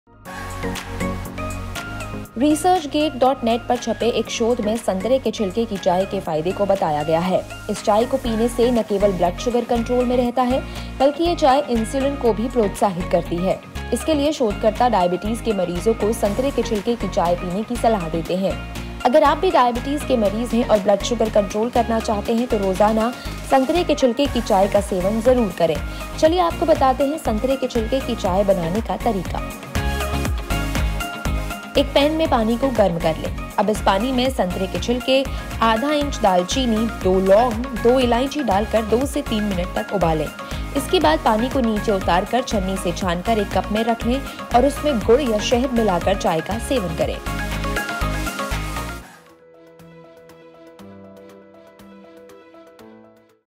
संतरे के छिलके की चाय नैचुरली करेगी blood sugar को कंट्रोल
संतरे के छिलके की चाय को पीने से न केवल ब्लड शुगर कंट्रोल में रहता है, बल्कि यह चाय इंसुलिन को भी प्रोत्साहित करती है.